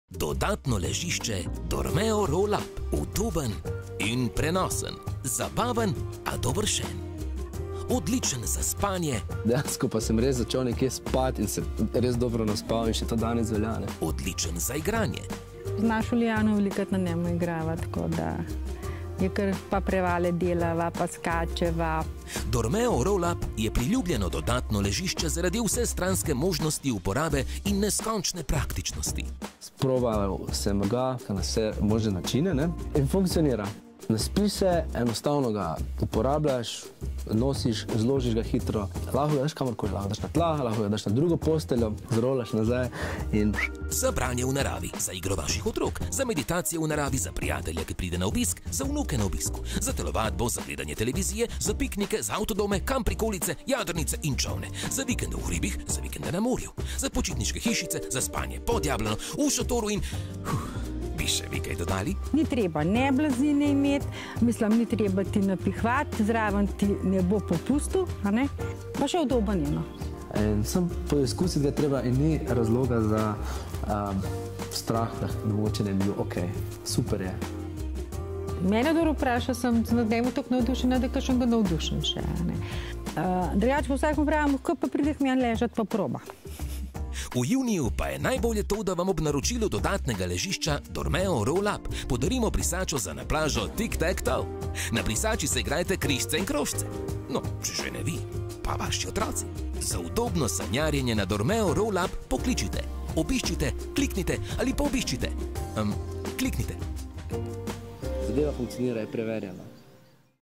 Dodatno ležišče Roll Up + darilo: brisača z igrico križci in krožci
V juniju kupcem Roll Up-a podarimo brisačo za na plažo z igrico križci in krožci.